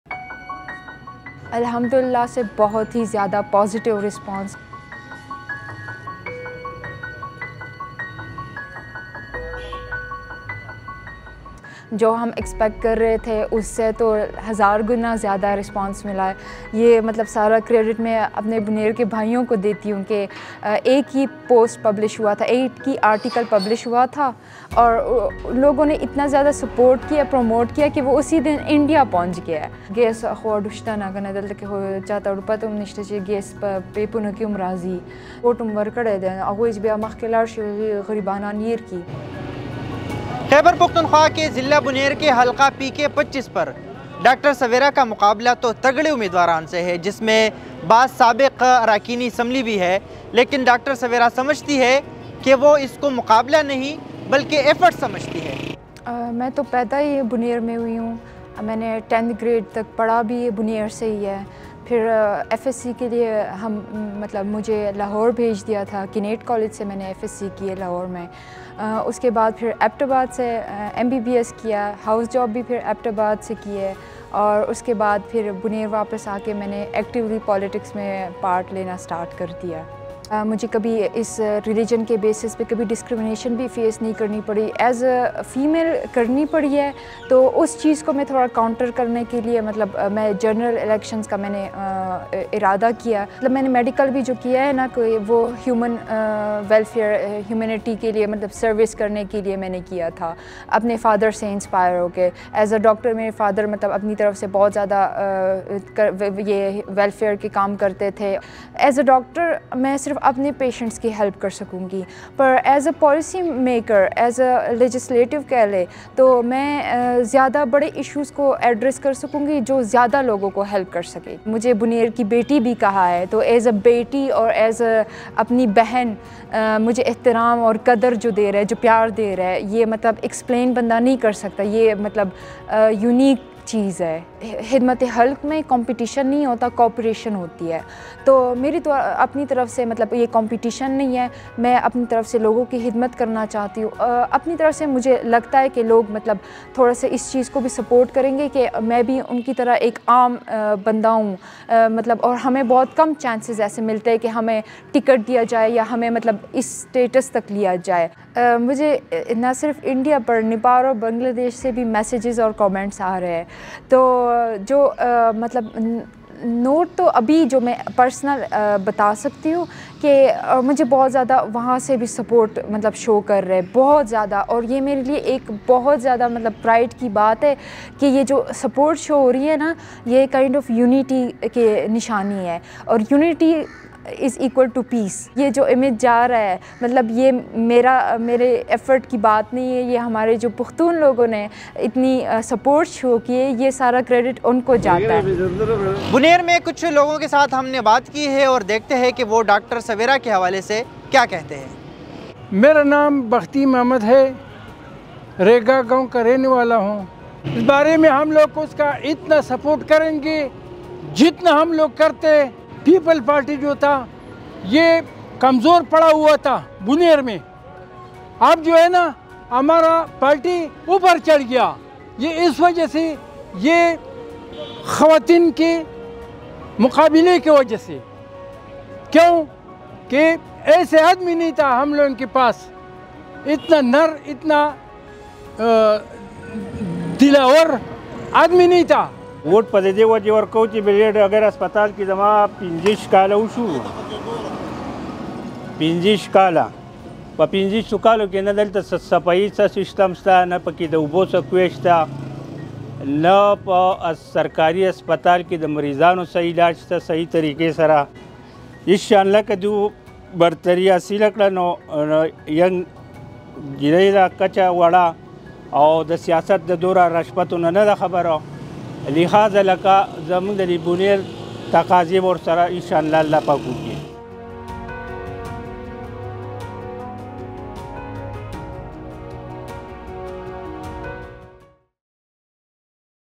Sawera Prakash, first Hindu woman to contest elections on a general seat from Buner
بونیر سے جنرل نشست پر انتخابات میں حصہ لینے والی پہلی ہندو خاتون سویرا پرکاش